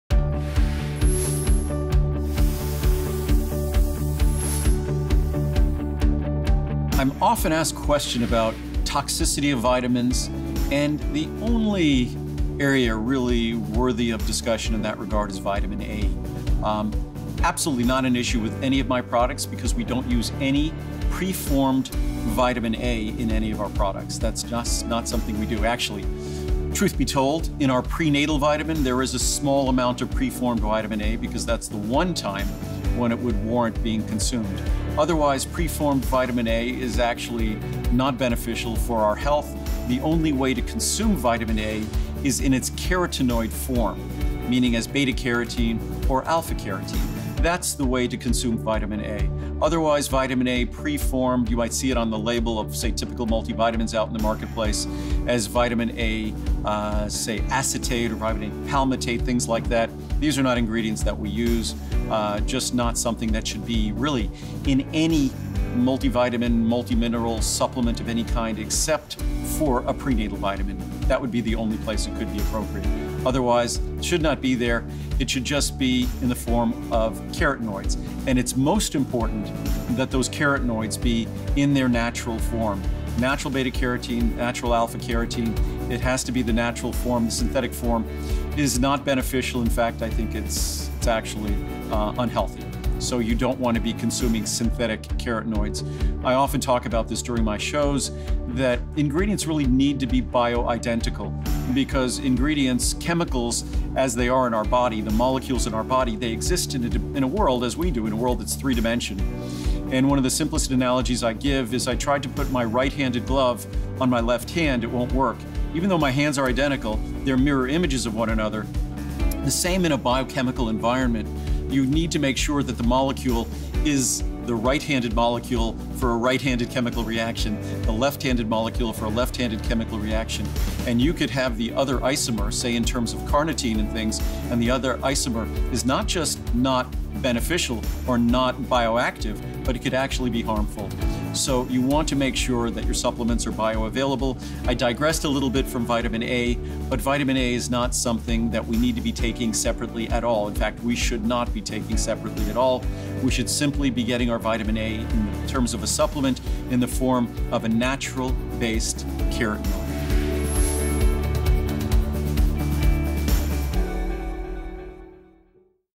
Should I be Taking a Separate Vitamin A Supplement?
Andrew Lessman discusses Vitamin A